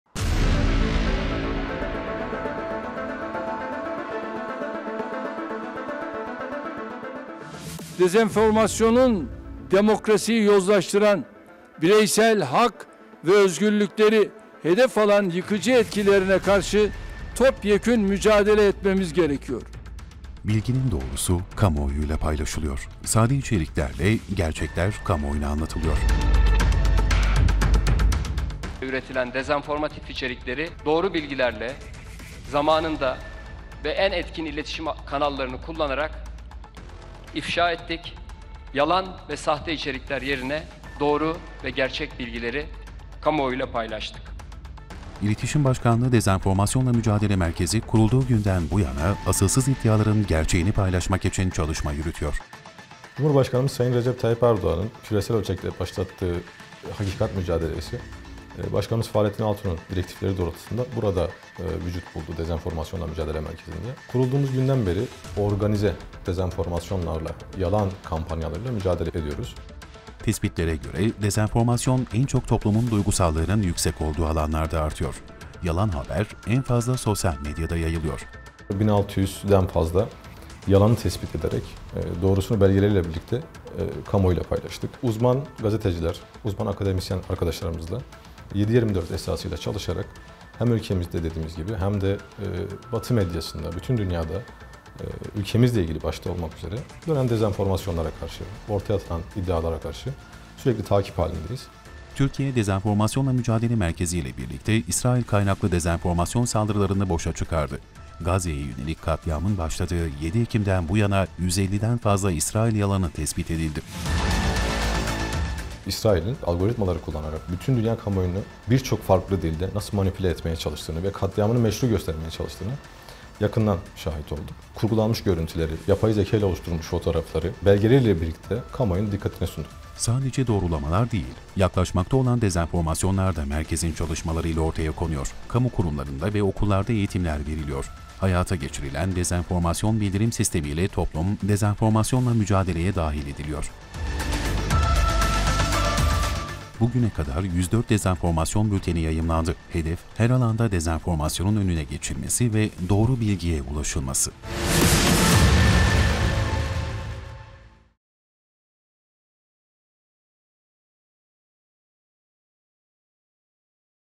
Dezenformasyonla Mücadele Merkezi, yalan içeriklerin önüne geçiyor
Uzman gazeteciler ve akademisyenlerin yürüttüğü çalışmalar ile toplumun doğru bilgiye ulaşması hedefleniyor.